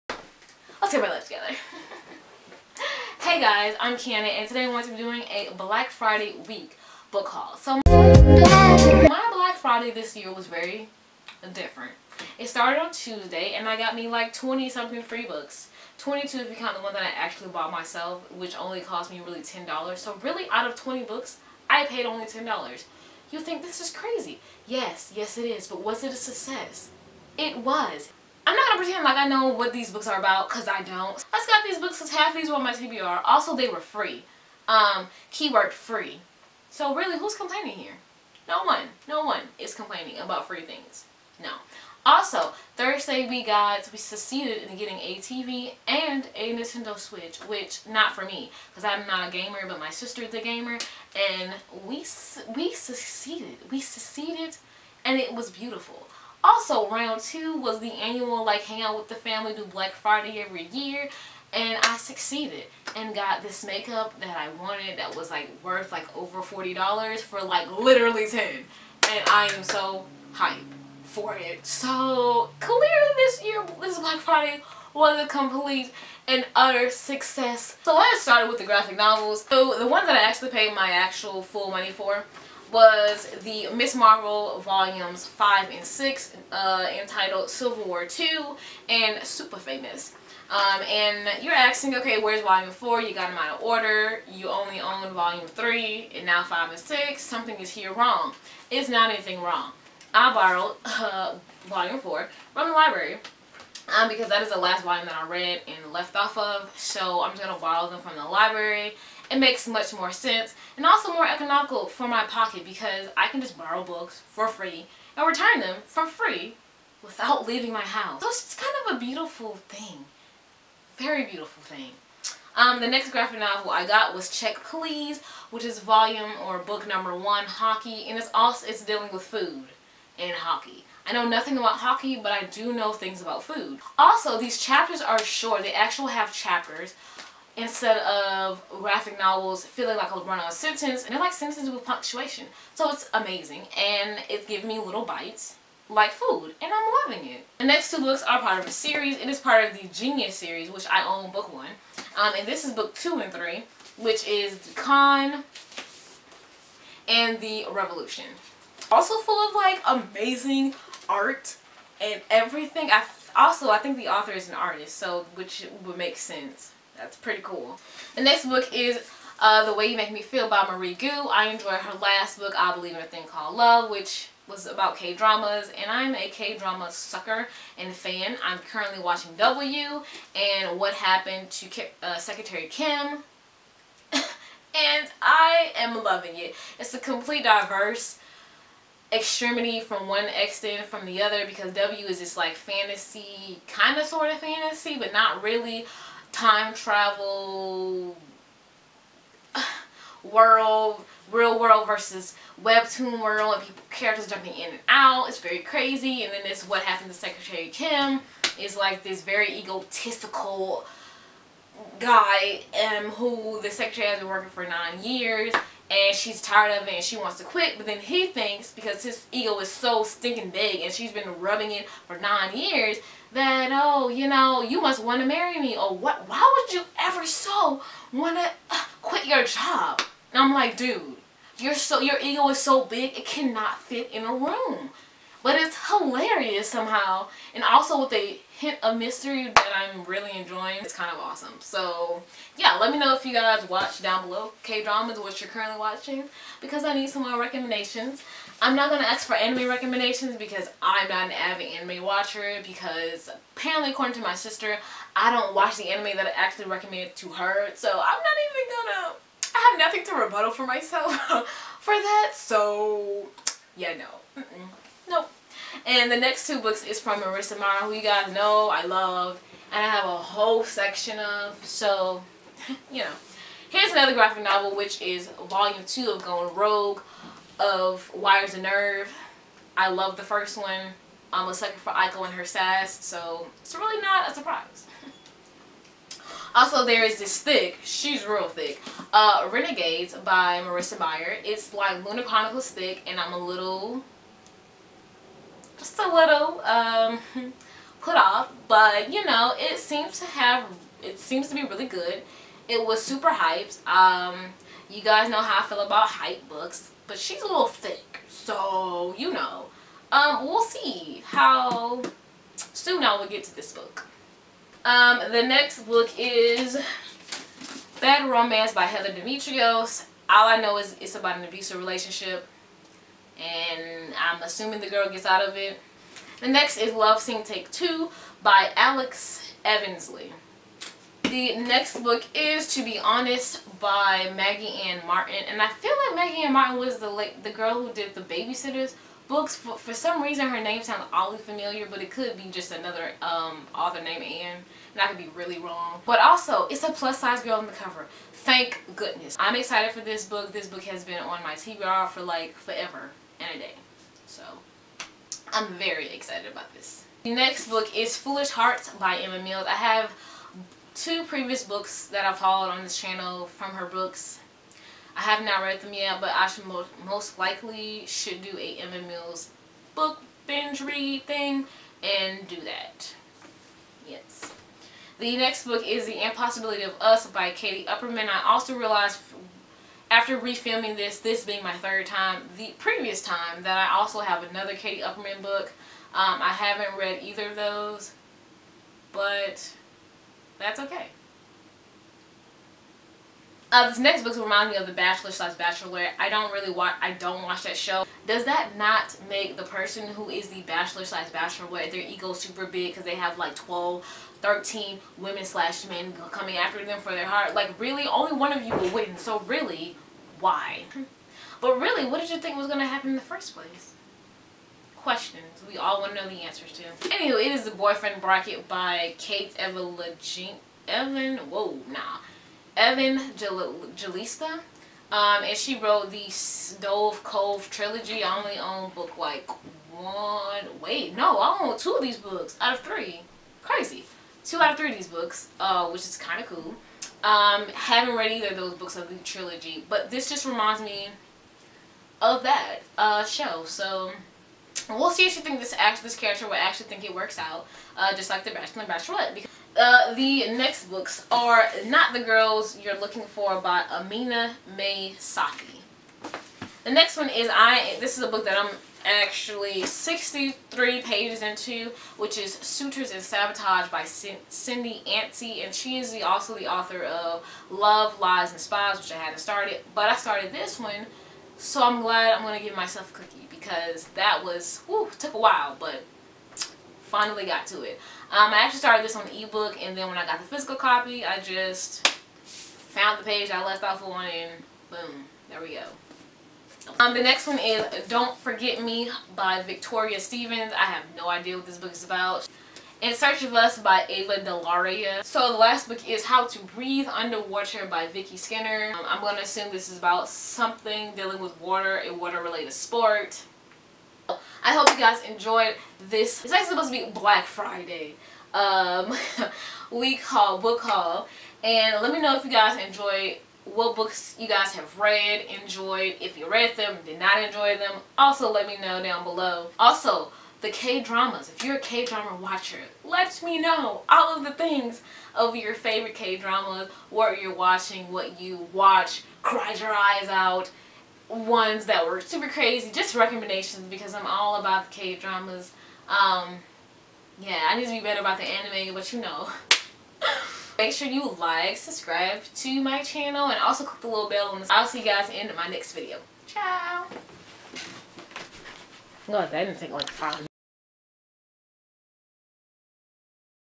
BLACK FRIDAY 2018 BOOK HAUL (OVER 15+ BOOKS) || KeyReadThat
In today's video, I hauled the books I received from my mom's boss and the two graphic novels I got from Barnes & Nobles.

//the socials //

love life, stay positive, + keep reading!

xo, k